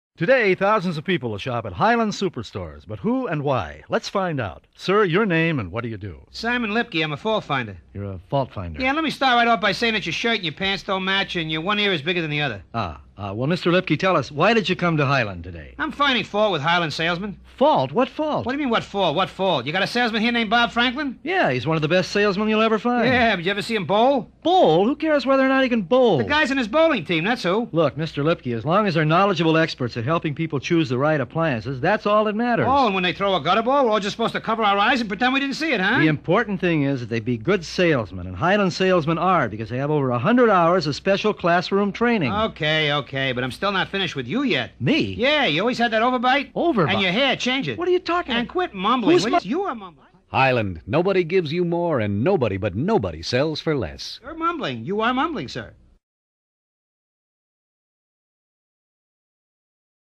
Highland Appliance Radio: Fault Finder
One of a series of terrifically-entertaining radio commercials created for Detroit-based Highland Appliance (later Highland Superstores) in the 1980s by W. B. Doner Advertising of Southfield, MI.